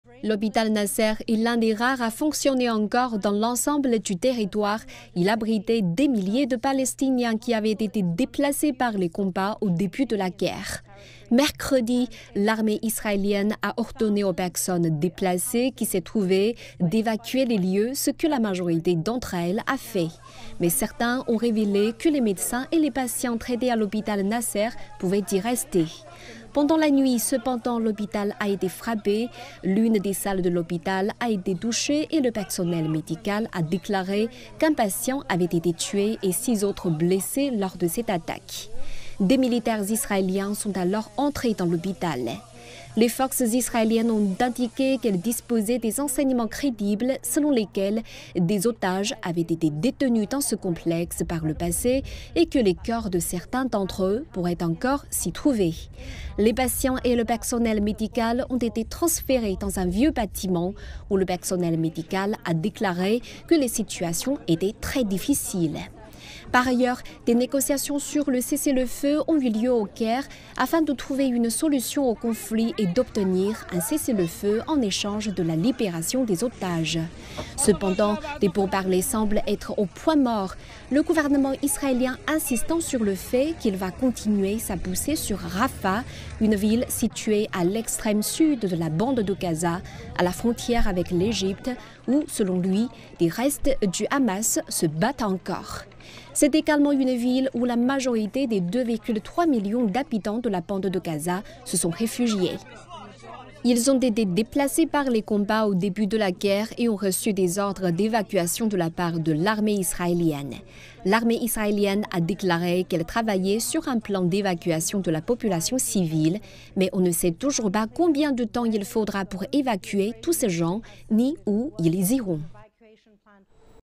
Les forces israéliennes lancent un raid sur l'hôpital Nasser à Khan Younès
Les forces spéciales israéliennes ont lancé jeudi un raid sur l'hôpital Nasser à Khan Younès, la deuxième ville de la bande de Gaza, dans le but, selon elles, de récupérer les corps d'otages israéliens. Plus de détails dans ce reportage depuis Jérusalem.